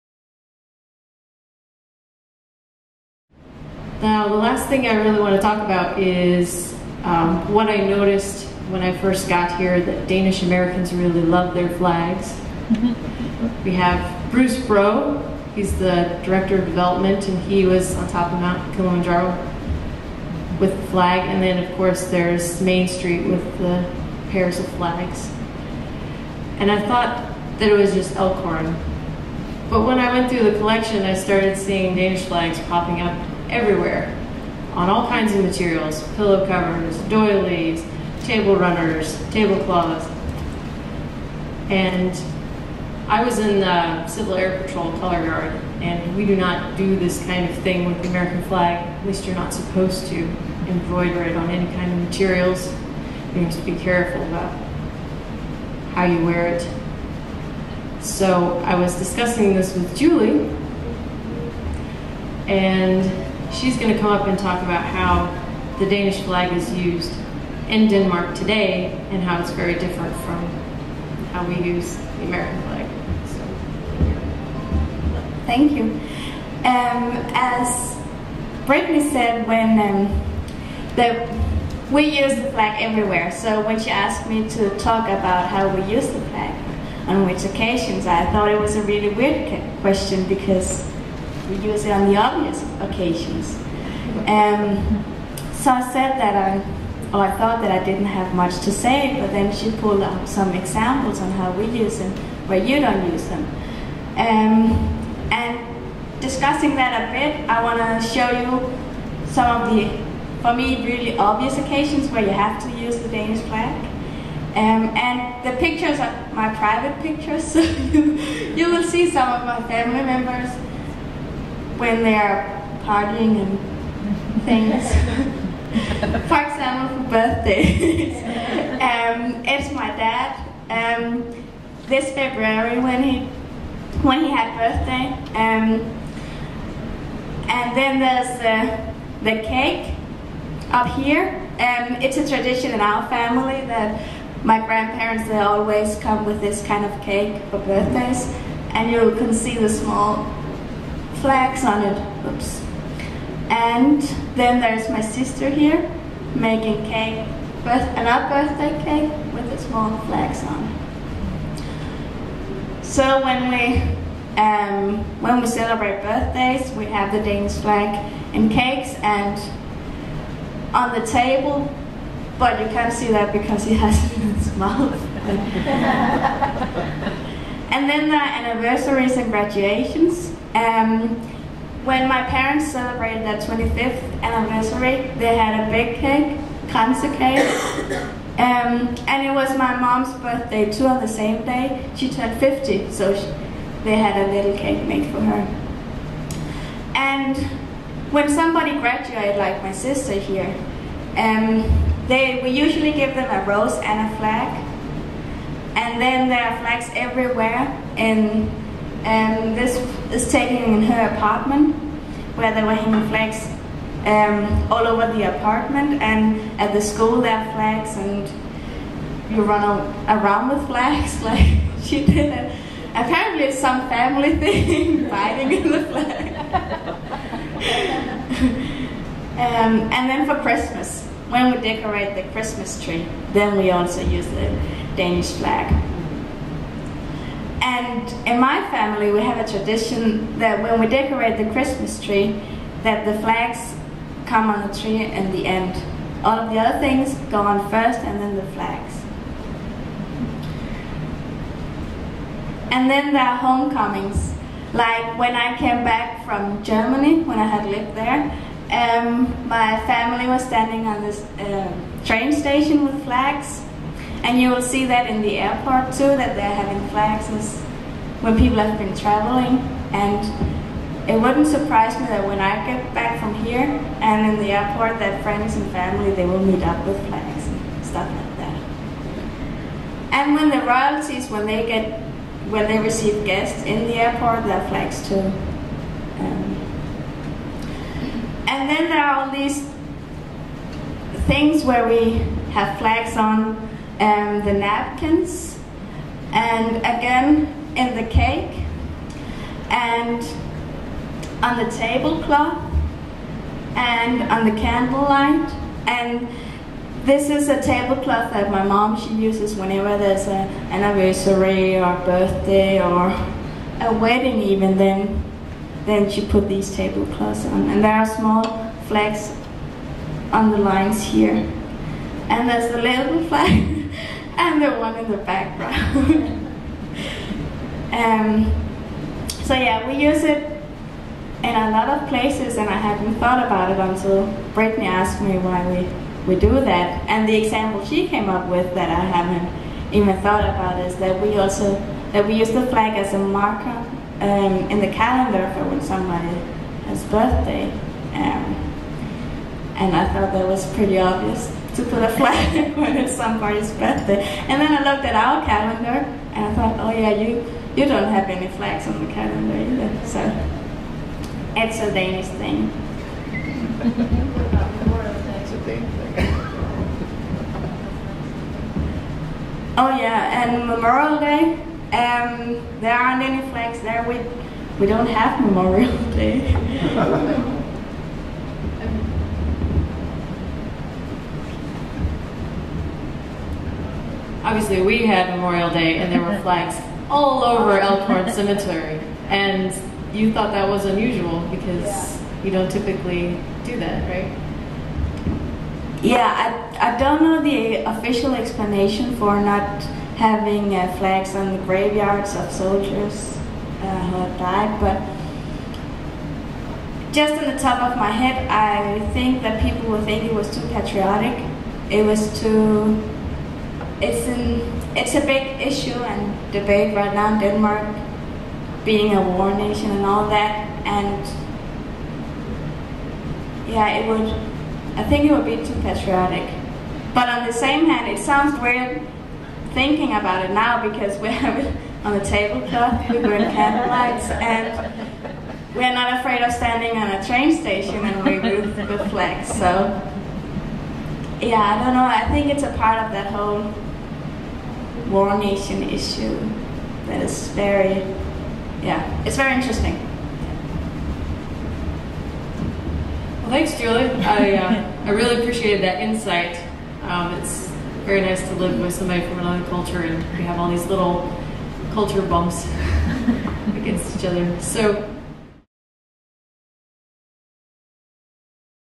A Common Thread Part 4
A Common Thread, Unraveling Danish-American, Stories in the Textile Collection. Danish flags in the textile collection by assistant Brittany Deeds & Use of Danish flags by Julie Andersen at The Danish Immigrant Museum